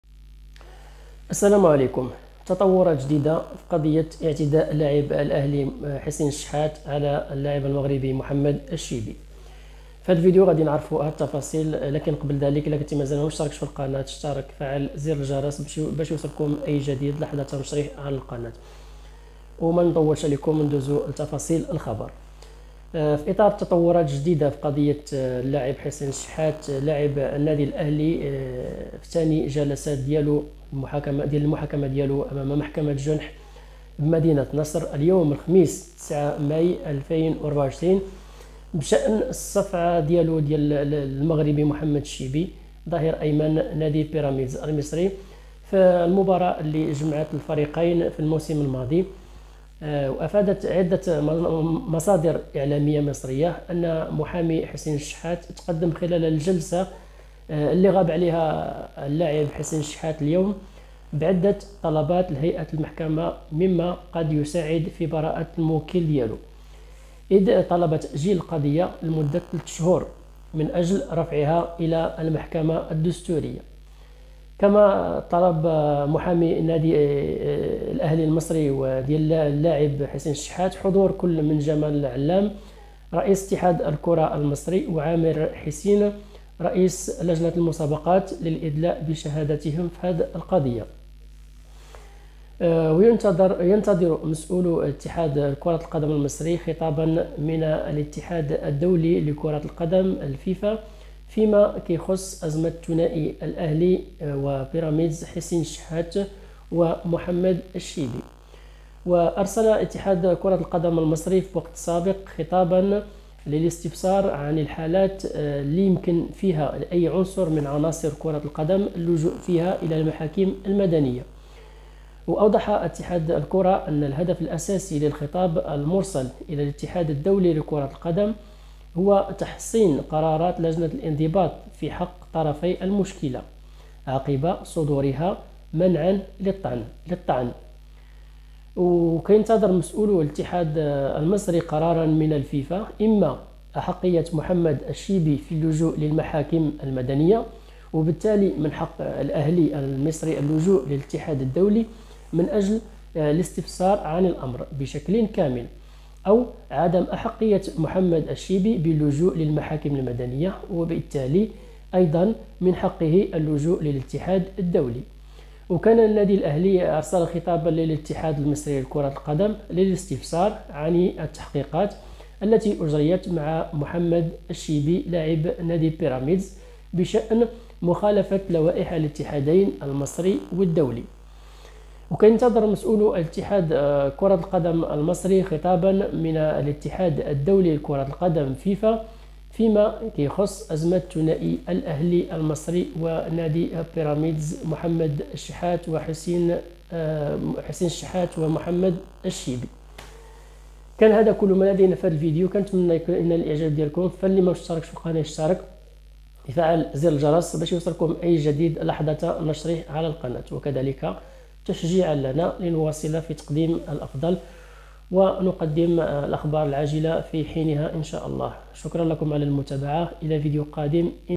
القضية كبرات بين الشحات والشيبي وتصعيد خطير من الإتحاد المصري ضد محمد الشيبي
الشحات
 الشيبي
الإتحاد المصري
لقجع
فوزي لقجع
المغرب
مصر
كرة القدم
قضية الشيبي